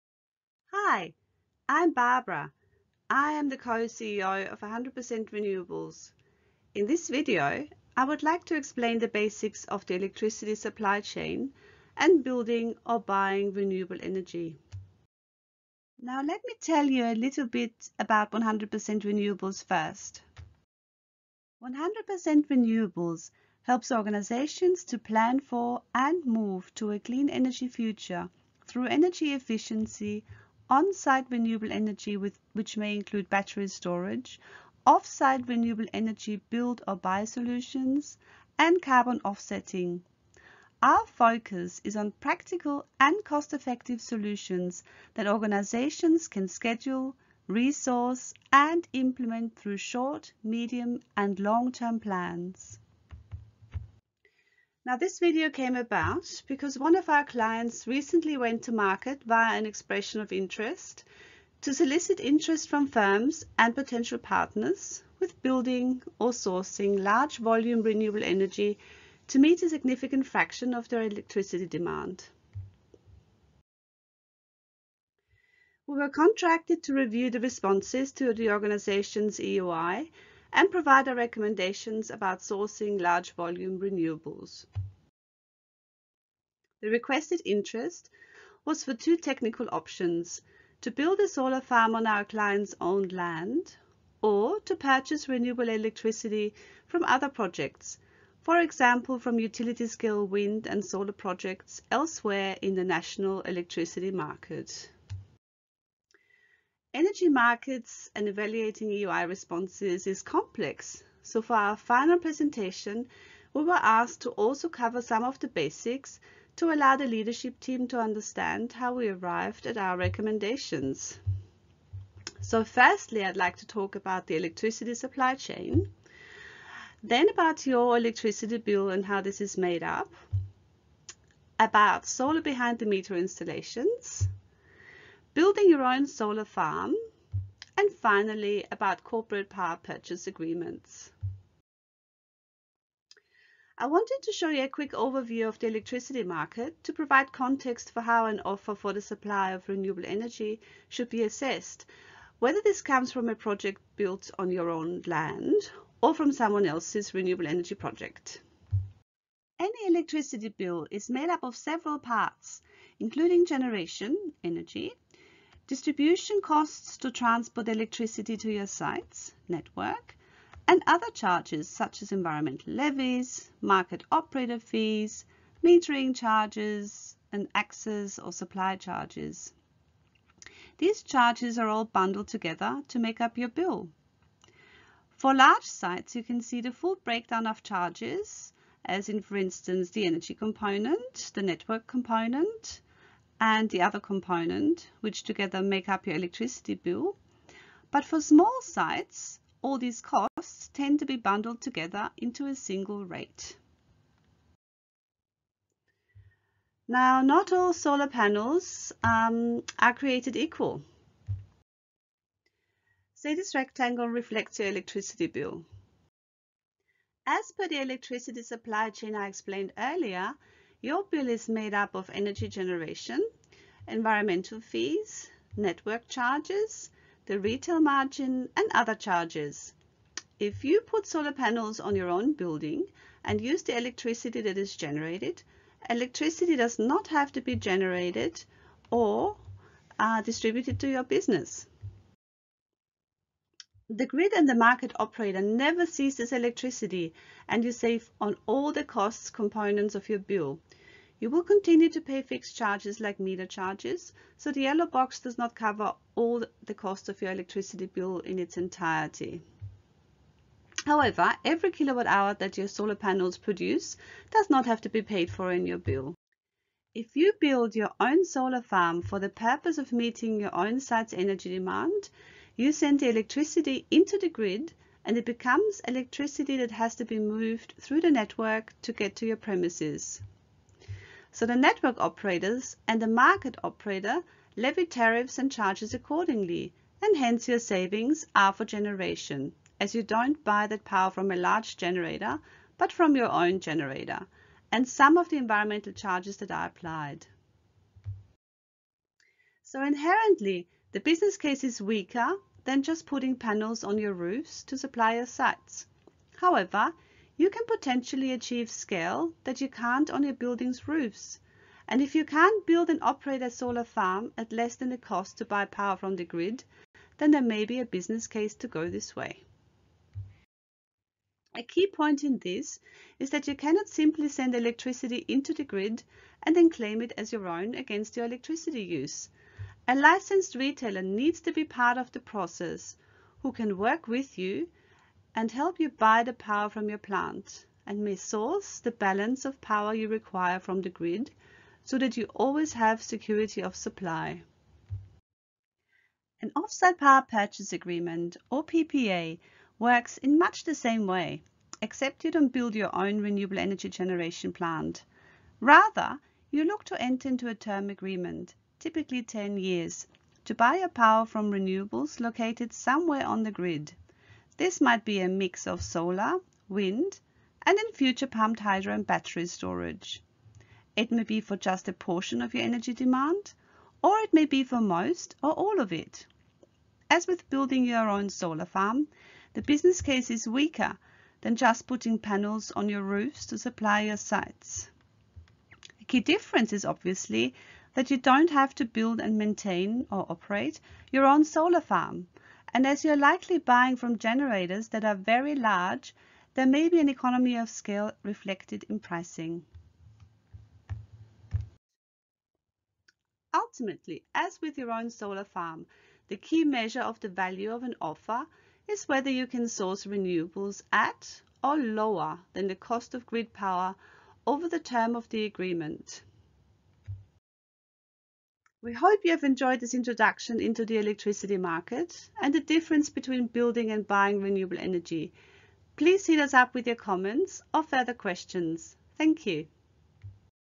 Barbara Albert explaining basics electricity supply chain build or buy large scale renewables
Barbara Albert, Co-CEO of 100% Renewables explains the basics of the electricity supply chain and what this means for building your own renewable energy project or buying large-volume renewables via a corporate Power Purchase Agreement.

SUMMARY:

- one of our clients went to EOI for sourcing large-scale renewables
- the EOI had two principal technical options: build your own solar farm or purchase renewable energy from an offsite project via a PPA
- explanation of the fundamentals of the electricity supply chain
- the components of your electricity bill
- difference between installing solar behind your meter versus building a large-scale solar farm, versus sourcing renewables from an offsite project.

RESOURCES:
You can find more valuable information on 100% Renewables' blog

Kind regards,
Barbara